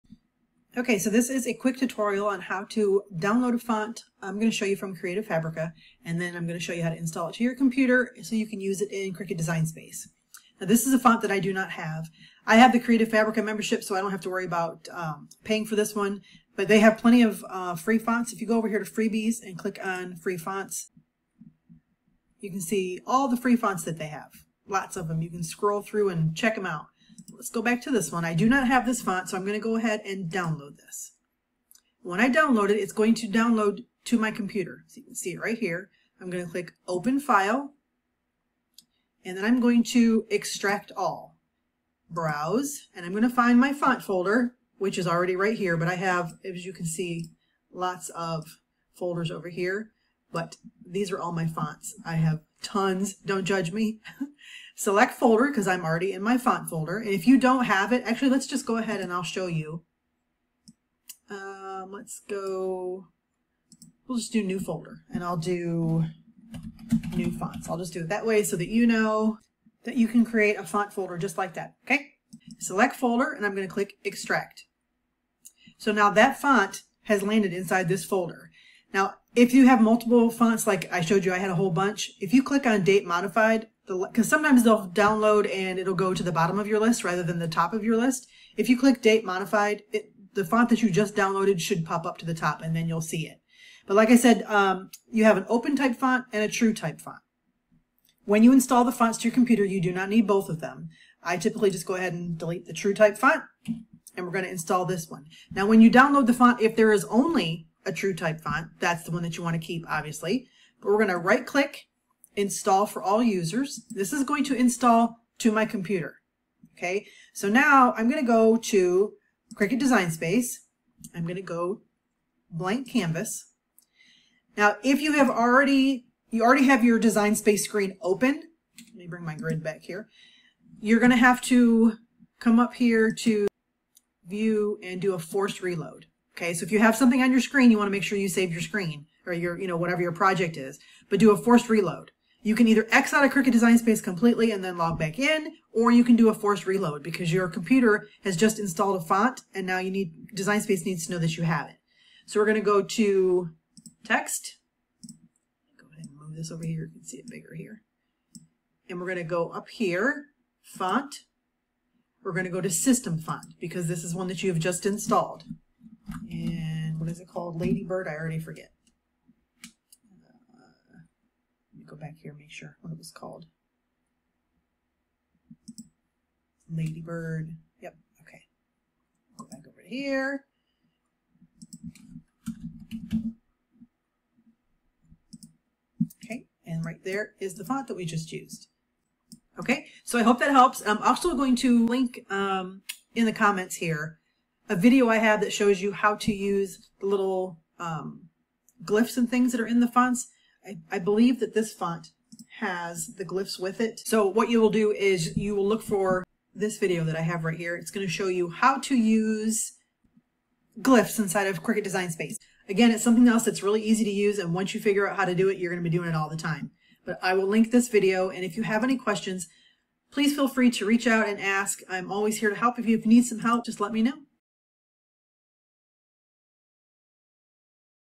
How to Download a Font from Creative Fabrica | Install & Use Fonts in Cricut Design Space
Learn how to download fonts from Creative Fabrica and use them in your Cricut projects! In this quick tutorial, I’ll show you step-by-step how to download a font, install it on your computer, and then open it right inside Cricut Design Space. Perfect for beginners who want to customize their projects with fun new fonts!

✍️ Want to use glyphs in your Cricut fonts?
(This video is from a couple of years ago, so your Design Space screen may look different now, but the functions are the same.)